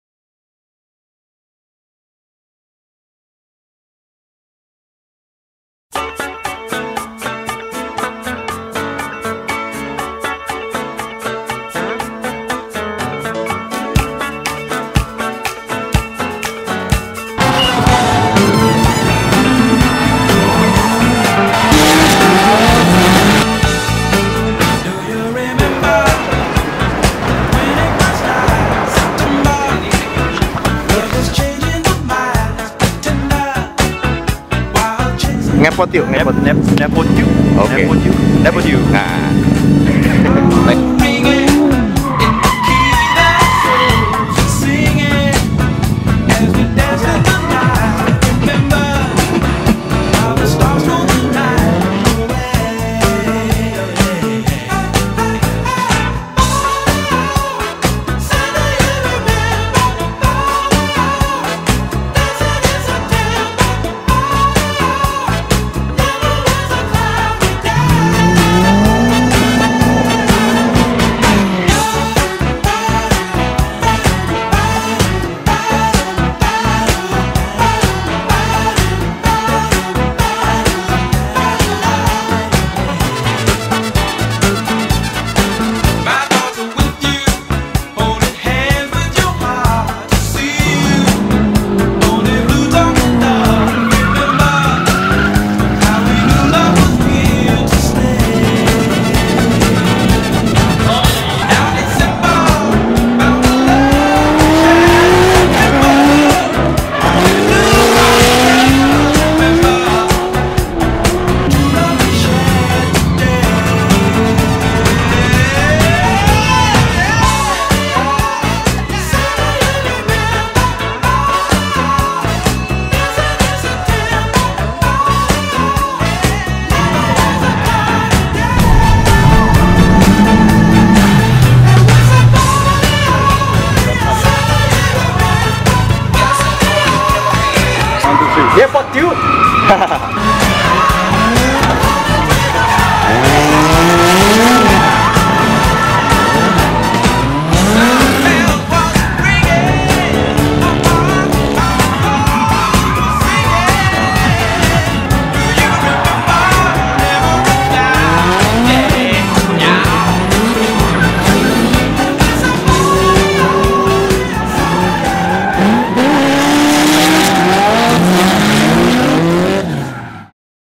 NGEPOTYUK : 9tro Drift Practice
"Ngepot yuk" is an Indonesian language slang, it means "Let's drift".
Ngepot yuk is a new program that will show you clips of drifting from around Singapore and Indonesia (and some other countries in the future).
This is my new video program that was inspired from Will Roegge's "Keep Drifting Fun".

Directed and Edited by Alrizki Marino

Camera operators :
Alrizki Marino
Lanang Bintang

Special Thanks to Lanang Bintang, Dwipurnama Mahendra, and Bernadeth AS (for the extra camera).